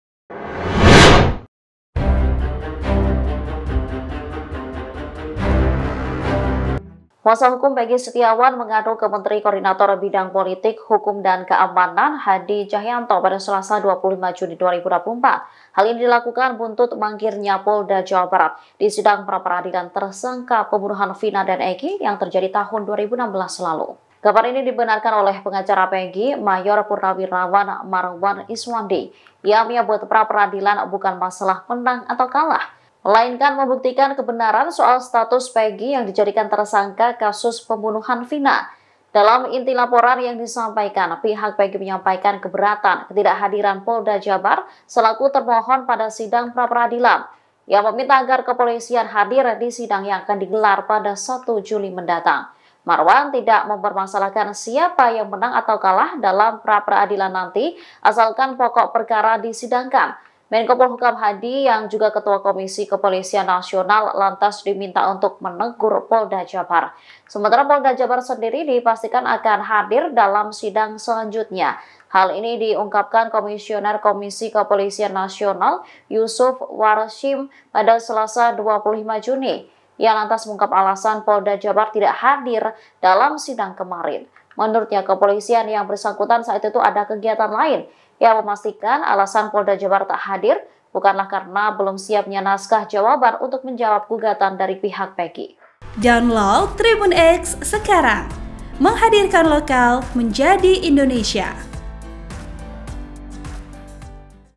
Mangkir Sidang, Kuasa Hukum Pegi Adukan Polda Jabar Ke Menkopolhukam
TRIBUN-VIDEO.COM - Kuasa hukum Pegi Setiawan mengadu ke Menteri Koordinator Bidang Politik, Hukum, dan Keamanan (Menko Polhukam) Hadi Tjahjanto, Selasa (25/6/2024).

Hal ini dilakukan buntut mangkirnya Polda Jawa Barat di sidang praperadilan tersangka pembunuhan Vina dan Eky yang terjadi tahun 2016 lalu.

Kabar ini dibenarkan oleh Pengacara Pegi, Mayor (Purn) Marwan Iswandi.

Ia menyebut, praperadilan bukan masalah menang atau kalah.

Melainkan membuktikan kebenaran soal status Pegi yang dijadikan tersangka kasus pembunuhan Vina.

"Ini kan (soal) menersangkakan, penahanan benar atau tidak. Argumen kami berbeda, argumen Polda Jawa Barat berbeda, makanya kami adu di praperadilan. Kalau di praperadilan sendiri dari Polda Jawa Barat enggak serius, ini bagaimana?” kata Marwan.

Dalam inti laporan yang disampaikan, pihak Pegi menyampaikan keberatan ketidakhadiran Polda Jabar selaku termohon pada sidang praperadilan.

“Poin intinya kami minta agar persidangan dari Polda (Jawa Barat) datang, hadir, ksatria. Kita kan untuk mengadu argumen,” ujar Marwan.

Ia meminta agar kepolisian hadir di sidang yang akan digelar pada (1/7).

Sumber Berita: Tribunnews
Editor Video : Sirajuddin Abbas
VO / Host :  Tri Suhartini